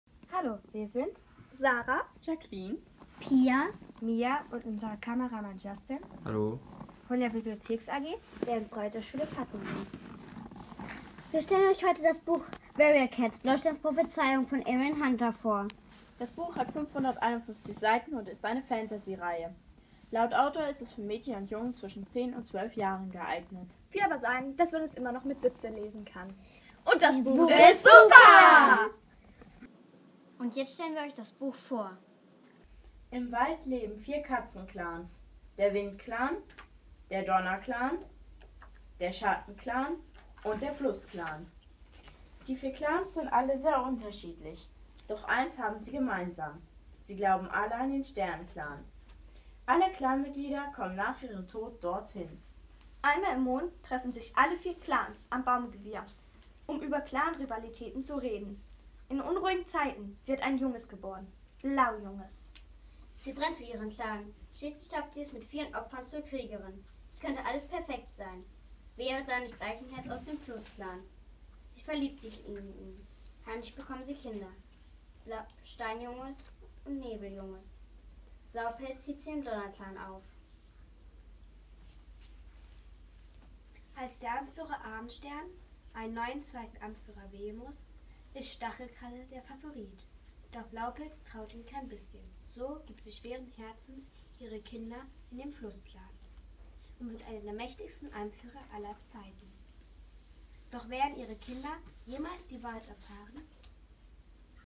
Ernst-Reuter-Schule Pattensen: "Warrior Cats" von Erin Hunter
Unter dem Motto „Bücher empfehlen – sag’s mit einem Video“ waren Schulen in Niedersachsen aufgerufen, eine Buchvorstellung und -kritik als 120-Sekunden-Film einzusenden. Die besten zwei Videos aus den Bereichen Grundschule und Sekundarstufe sowie das Video mit den meisten Klicks bis zum 12. September 2016, 12 Uhr bei YouTube werden am 28. September 2016 beim Niedersächsischen Schulbibliothekstag in Hannover prämiert.